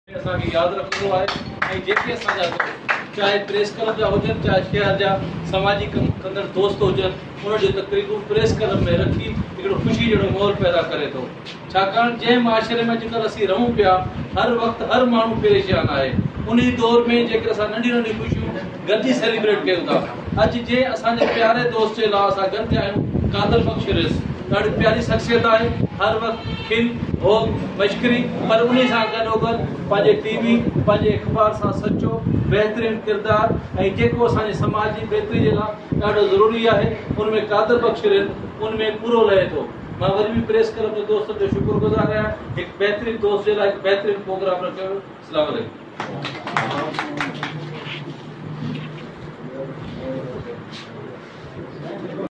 Speech
Abdul Hameed Arain Chairman CDF-Pakistan speech during the program birthday 🎊 party of Qadir Bux Rind journalist of Sakrand arranged by press club Sakrand at press club Sakrand